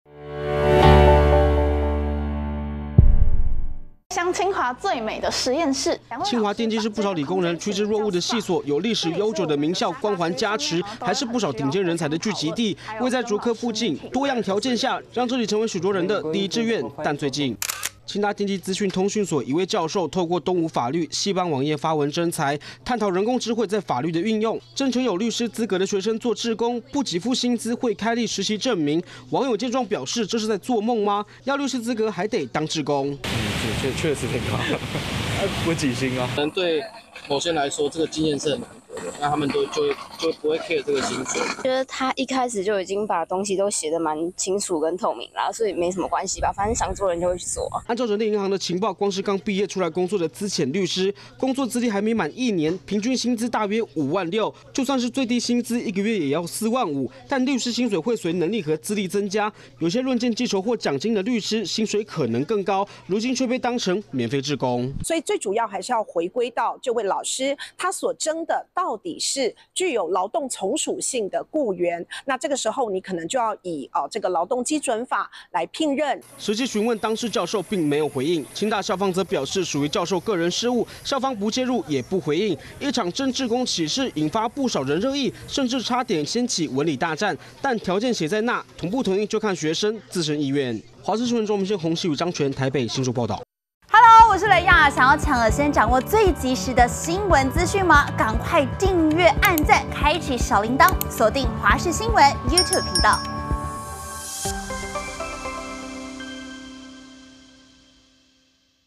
徵「律師資格」志工惹議 清大當事教授未回應｜華視新聞 20240618
清大電機一直都是理科生的熱門選擇，近日網友在社群軟體上分享，清大電機資訊學院通訊工程所，1位教授正在徵才，要找到5名具有律師資格，兼職或全職的長期志工，探索人工智慧在法律領域的應用，雖然可以遠端和開立實習證明，但完全不給薪，文章一發引起熱議，清華大學則表示，事件屬於教授個人事務，校方不介入，而當事教授則沒有回應。